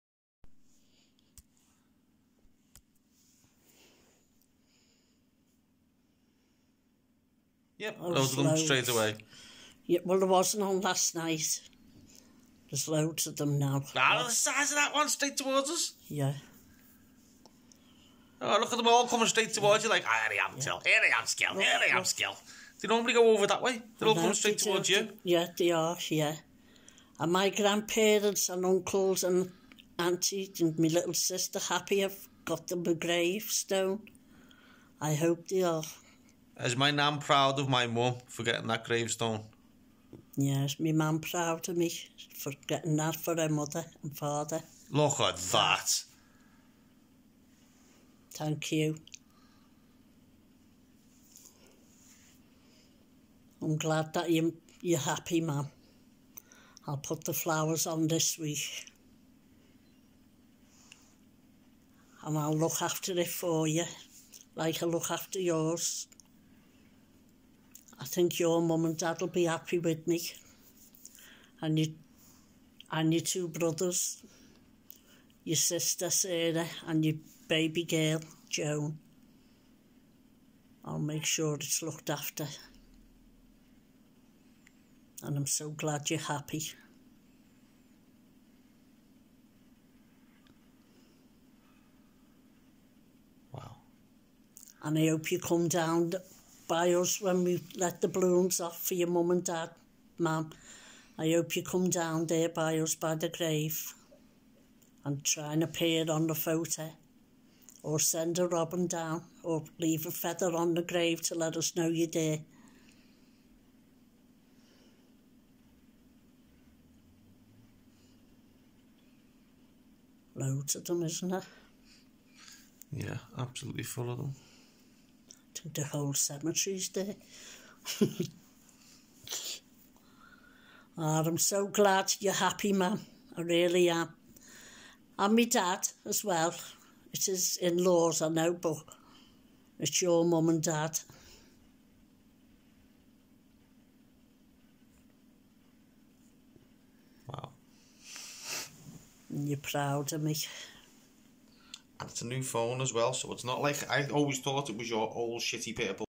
Spirit activities , Cathy Skel
Skelys,.. I asked my deceased mother was she happy I have bought her mother and father a headstone, what was put on the grave 26/4/21 after 76 years unmarked x